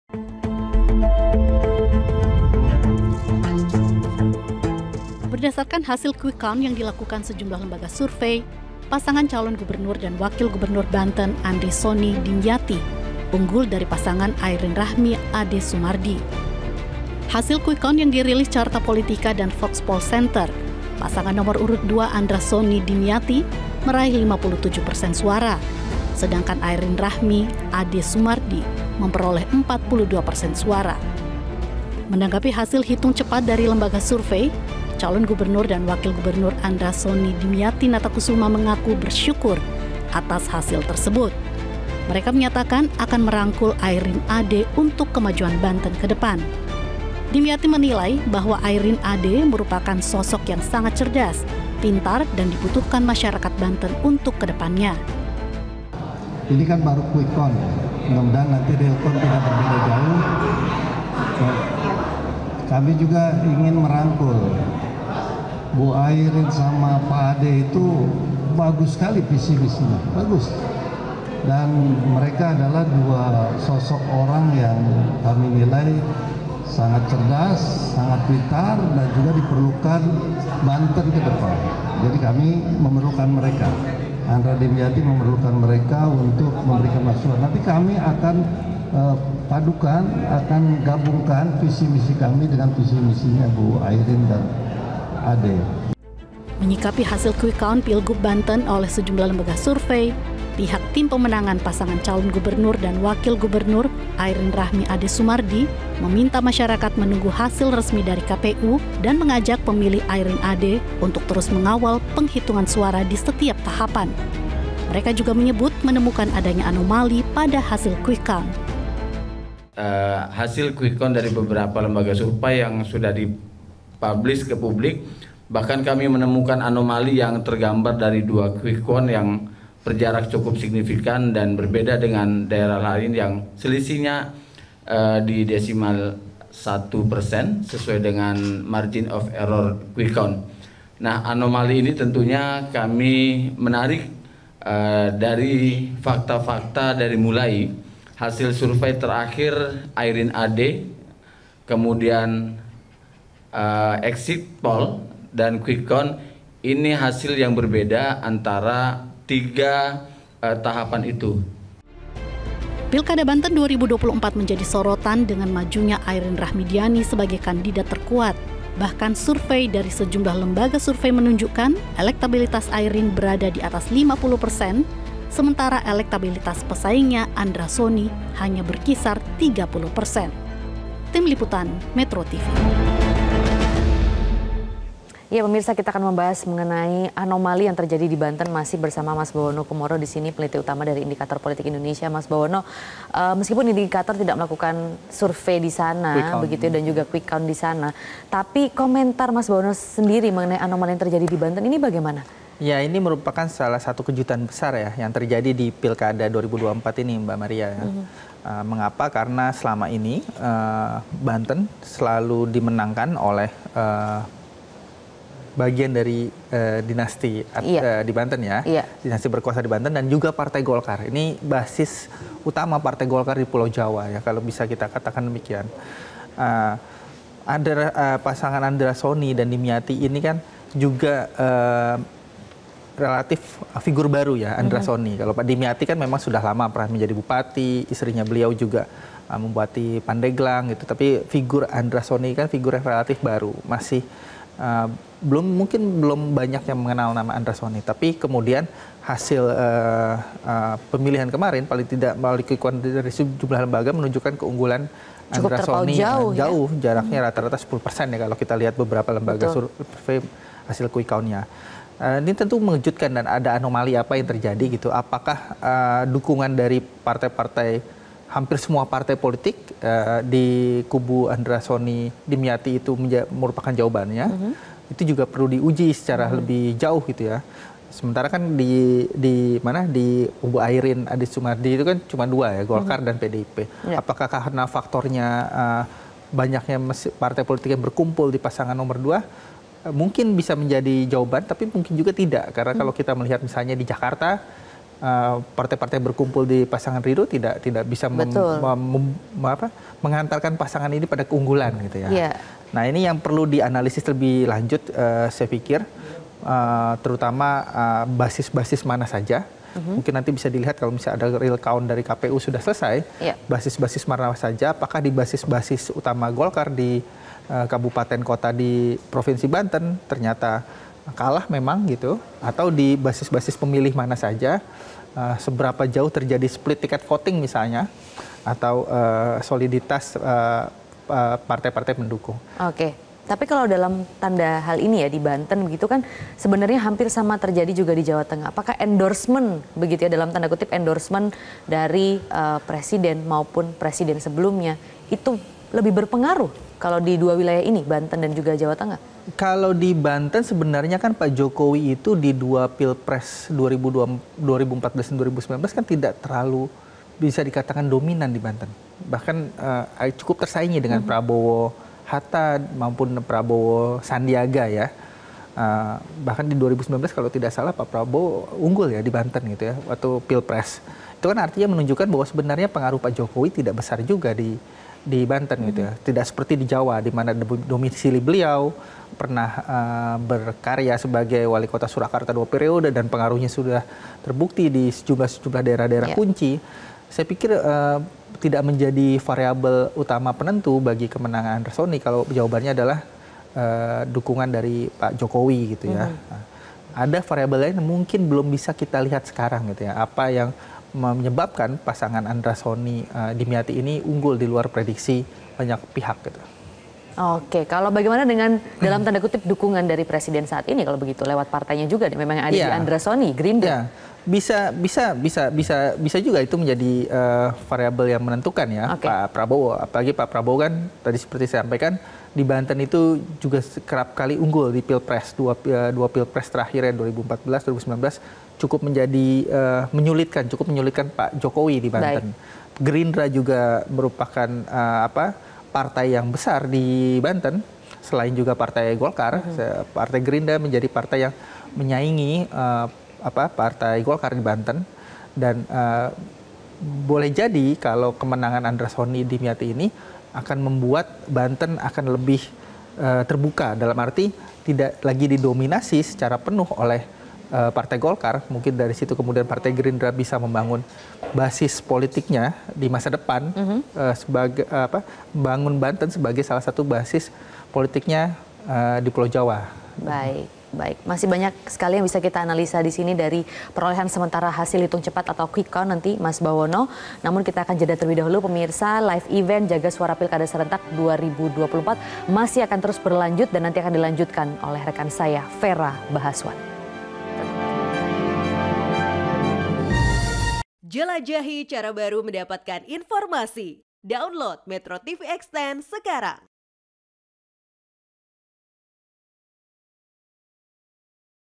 Penghitungan Suara Pemilihan Gubernur Banten
METRO TV -  Penghitungan Suara Pemilihan Gubernur Banten, Hitung Cepat Pilkada Banten, Airin Kalah?